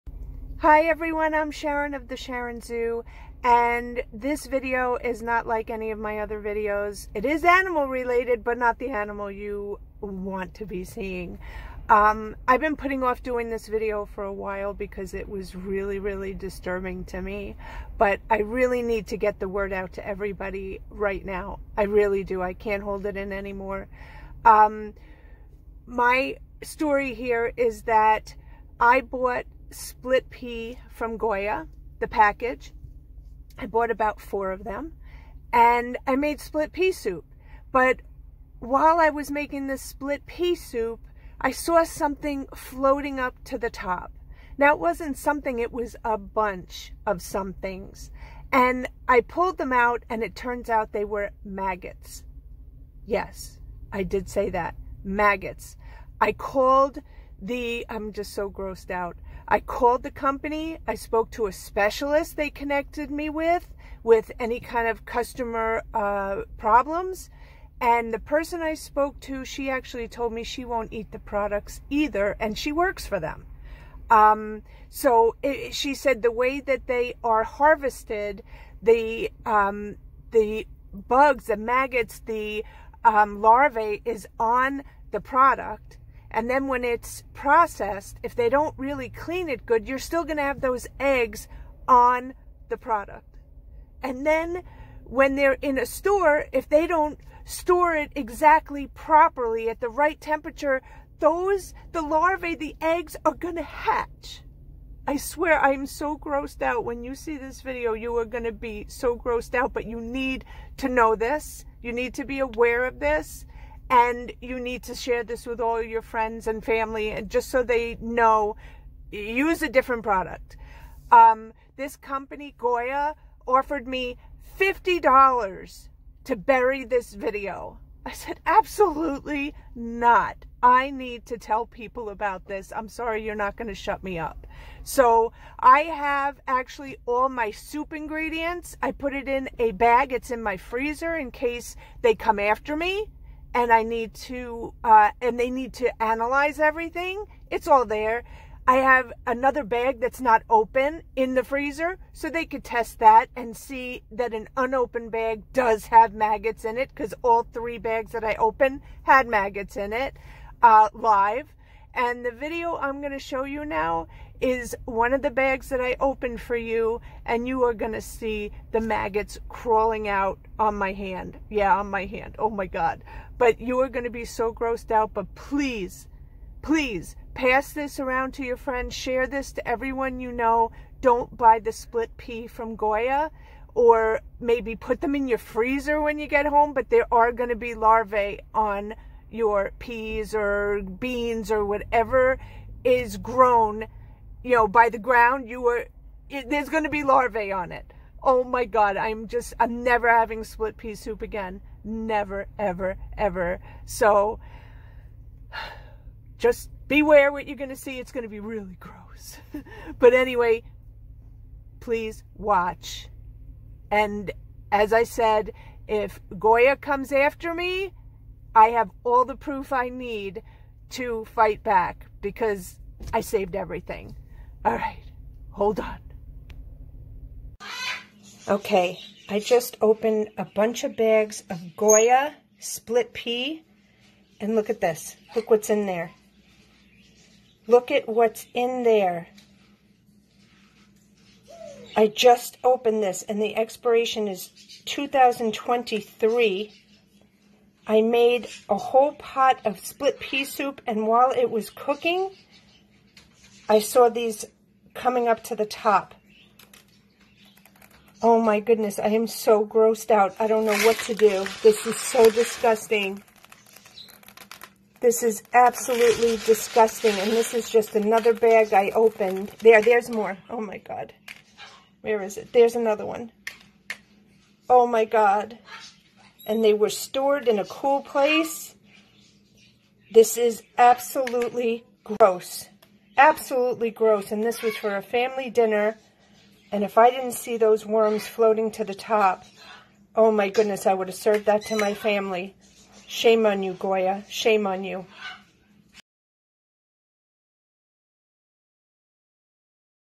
BEWARE MAGGOTS In GOYA Products| Video Proof In This Video
I bought 4 bags of Goya Split Pea & proceeded to make soup for my family. To my HORROR MAGGOTS were floating to the top of the pot😖 I called Goya Consumer Affairs and spoke with a specialist. I proceeded to tell her my story and she offered me $50 to GET RID of my video, but I refused! I have the emails to show how much they offered me and that I would have to sign a contract to NOT POST the video! She proceeded to tell me that she has worked for the company for a while and she knows how they harvest their products & what goes on in the processing plant and she refuses to eat it herself because of this issue. I saved all the contents of my soup and stored it in my freezer and I saved an unopened bag and also stored that in my freezer. I have it stored just in case Goya wants to cause a problem for me for posting the TRUTH!
PLEASE SHARE AND MAKE EVERYONE AWARE!

Welcome to the home of Sammy the Famous Mini Pig, his Bird Friends and Crazy Cat sisters!